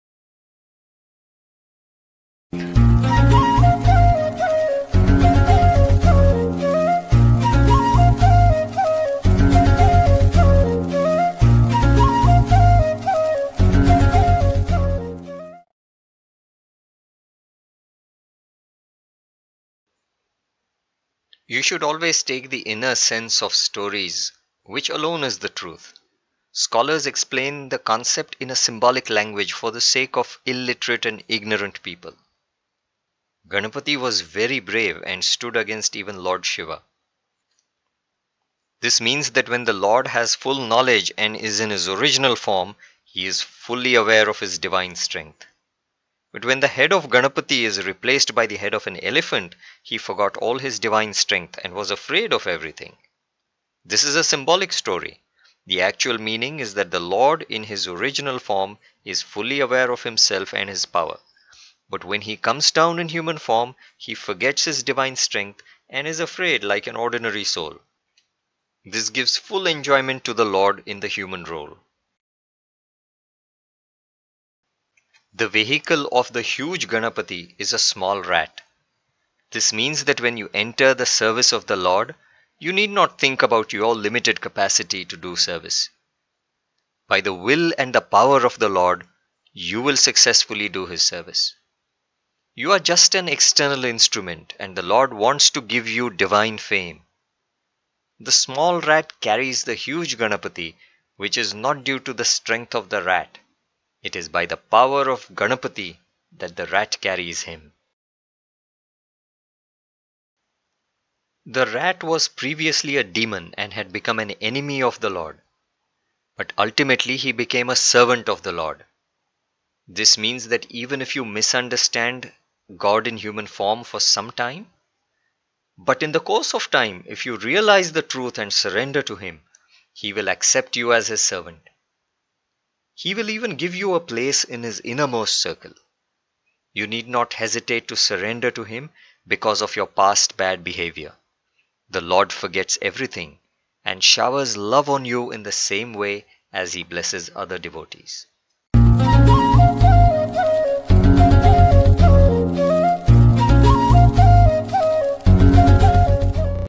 Part10 - Significance of Ganesha Riding a Mouse
GANESH CHATURTHI MESSAGE
Part 10 - The Significance of Ganesha Riding a Mouse
Why does Lord Ganapati ride a mouse?
Why did the Lord accept a demon as His servant?

“The Lord forgets everything and showers love on you in the same way as He blesses other devotees.”

This video is part of 12-part playlist of videos on the Divine Discourse given by His Holiness Shri Datta Swami.

Ganesh Transparent , a href=" Transparent FreePNGImg.com/a
Lord Shiva, Ganesha and Parvathi [Raja Ravi Varma [Public domain]]
Lord Shiva and Markandeya (Raja Ravi Press [Public domain])